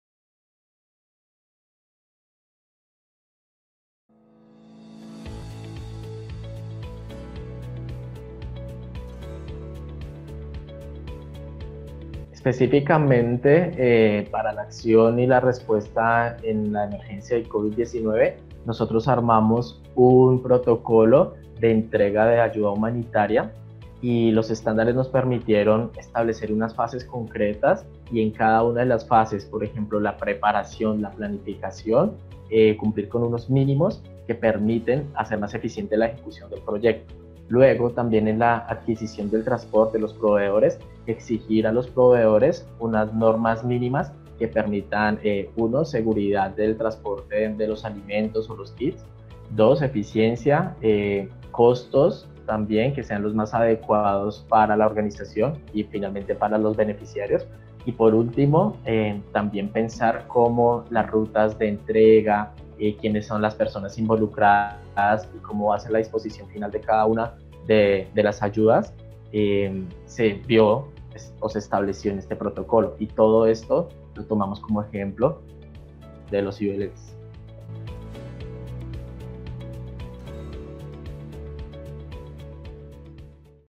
ULSQuestions - Why ULS Standards are important in Covid-19 times? - Jonathan David Muñoz
Jonathan David Muñoz, from TECHO, explains how ULS have helped his organization to carry out their work during COVID19. A group of 15 logistic experts have contributed to this project led by the Inspire Consortium (Groupe URD (Urgence Réhabilitation Développement), Instituto de Estudios sobre Conflictos y Acción Humanitaria (IECAH), Global Public Policy Institute (GPPi) and Overseas Development Institute (ODI)).